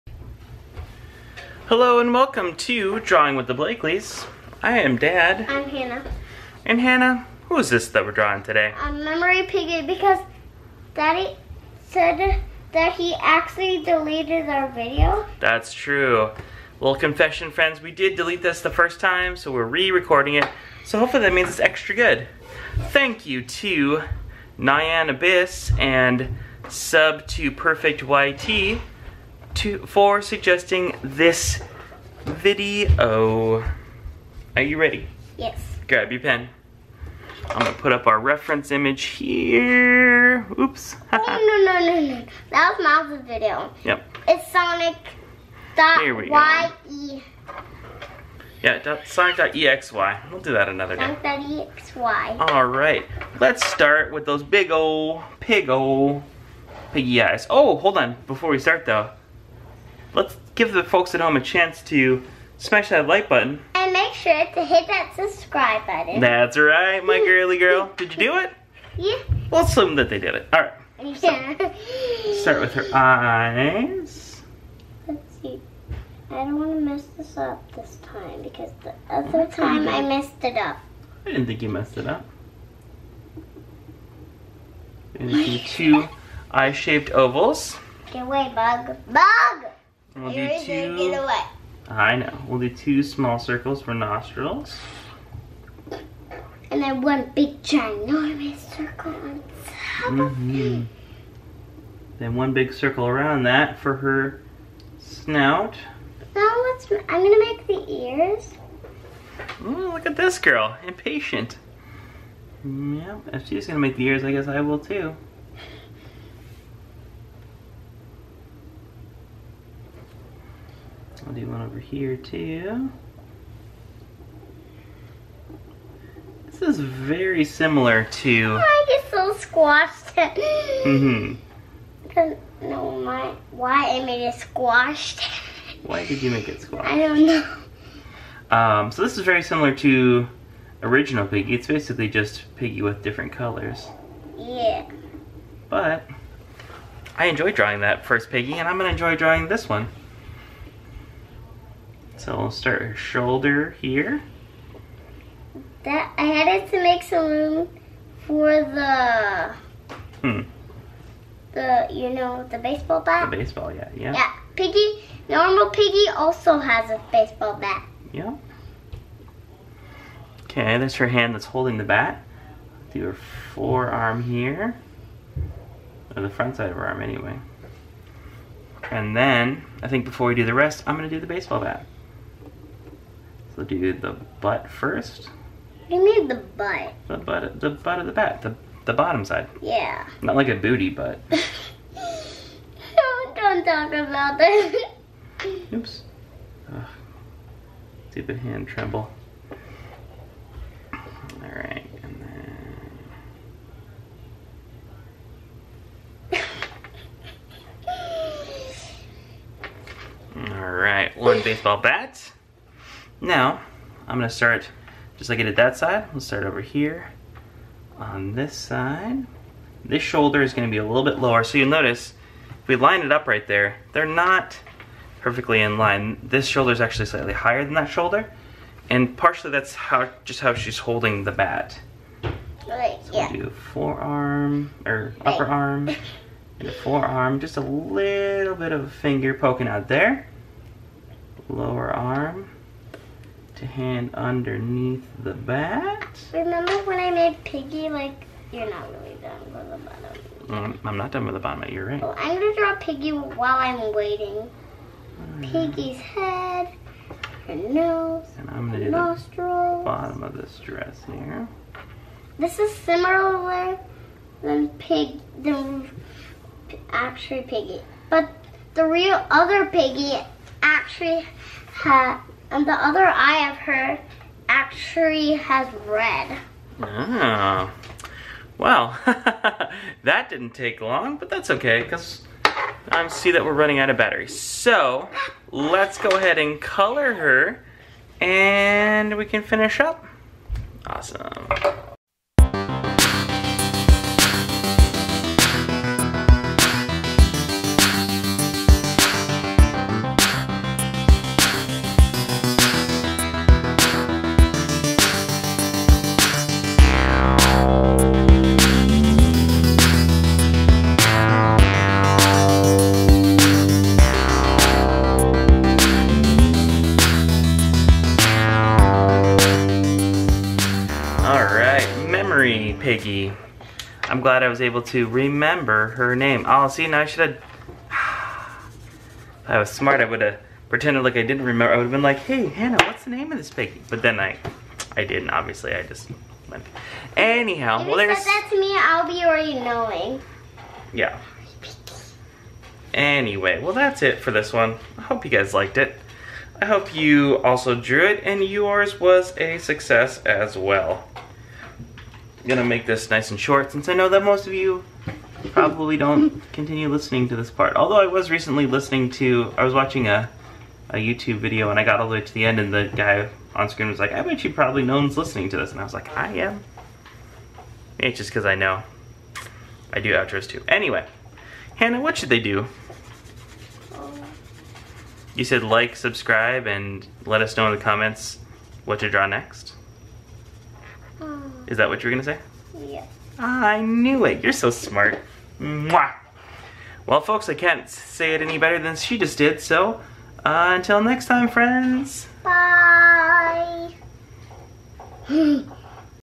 How to draw Memory (Piggy)
This time, Dad and Hannah drew Memory from the Roblox game Piggy.  Thank you to Sub to perfectYT and nyan abyss for suggesting this drawing.  If you want to tell the Blakleys what to draw next, leave a request in the comments.